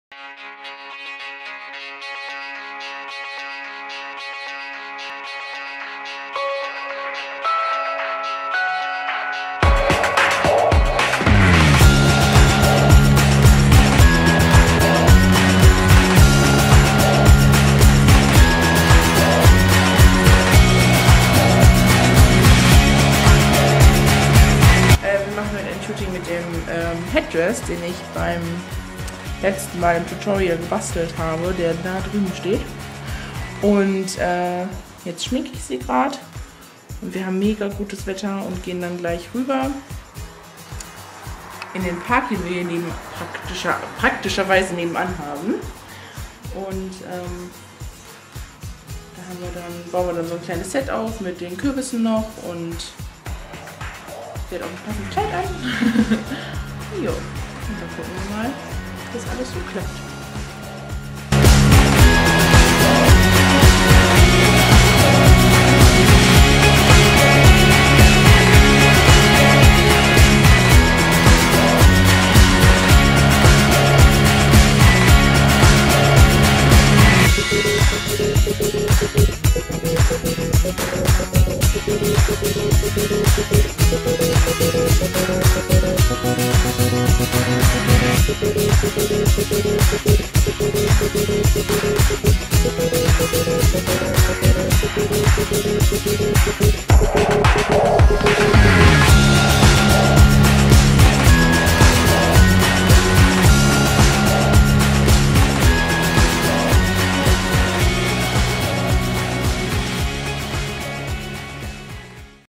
Making of Pumpkin Queen
In diesem Video nehmen wir euch mit in unser Shooting mit der hübschen Linda!

Mit dem gebastelten Kopfschmuck, ein paar Kürbissen und einer Laterne konnten wir eine tolle Kulisse erschaffen!